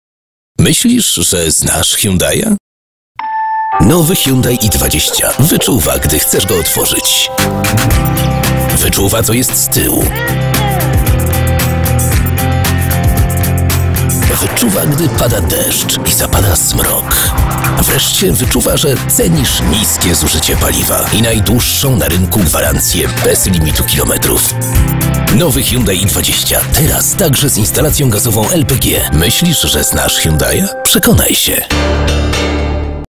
Nowy Hyundai i20 - teraz także z LPG! | Grupa Gezet
Nowy Hyundai i20 dostępny także z fabryczną instalacją LPG!